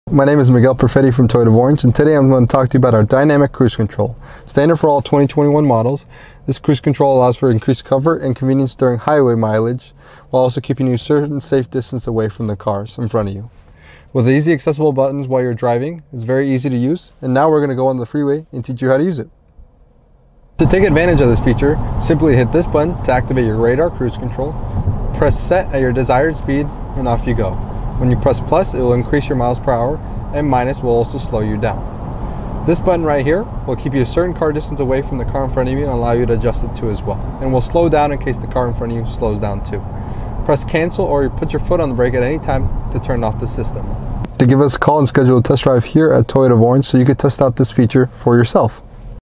Toyota of Orange Presents: Dynamic Cruise Control with Miguel!
Happy Friday! Miguel is back to show you how to use the Dynamic Cruise Control available in select Toyota models! Come down today for a test drive and use this feature!